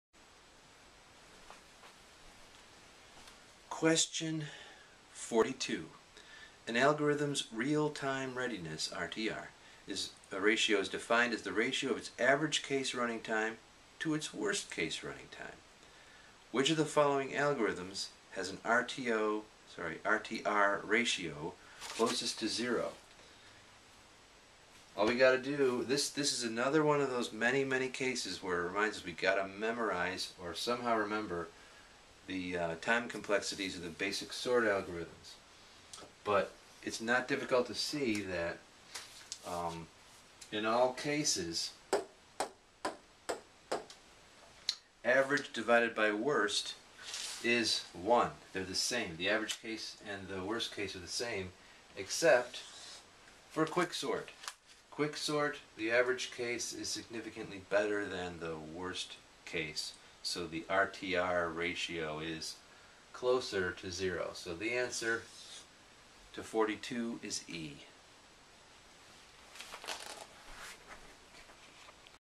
GRE Computer Science Question 42
42. An algorithm's real-time readiness (RTR) ratio is defined as the ratio of its average-case running time to its
worst-case running time.
Which of the following algorithms has an RTR ratio closest to 0 ?
(A) Bubblesort (B) Heapsort (C) Insertion sort (D) Mergesort (E) Quicksort